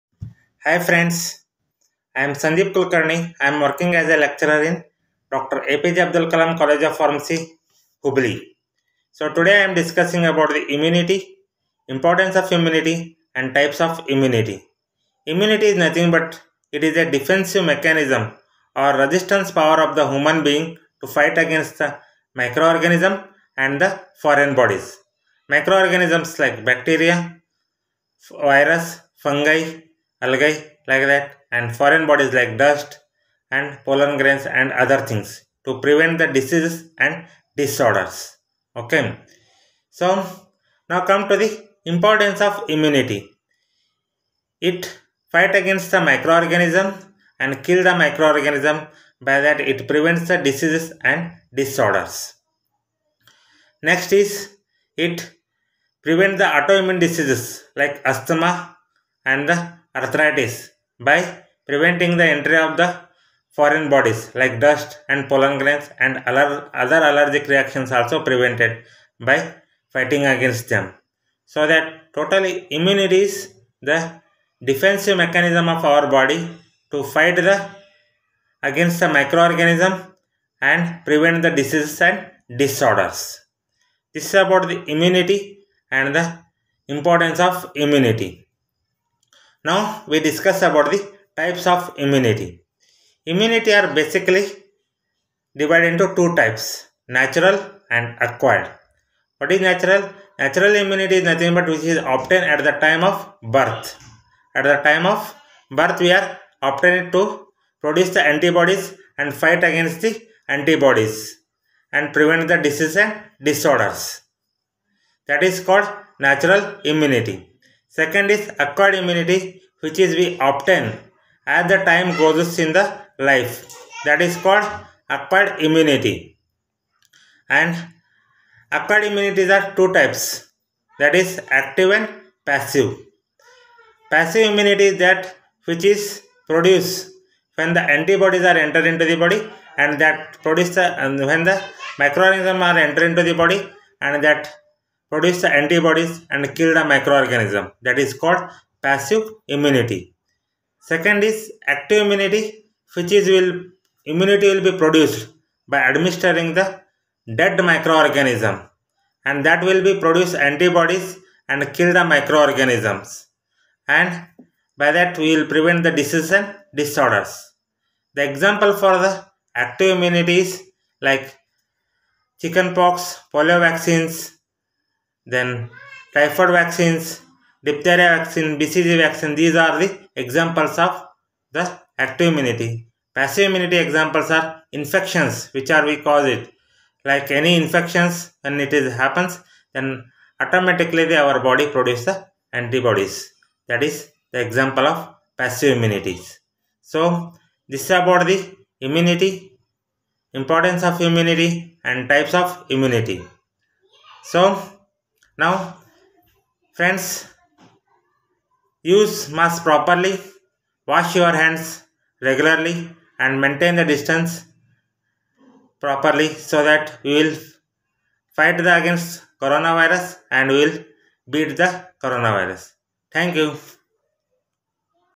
IMMUNITY INFORMATION BY SANDEEP KULKARNI LECTURER, DR APJ ABDUL KALAM COLLEGE OF PHARMACY, HUBBALLI, KARNATAKA, INDIA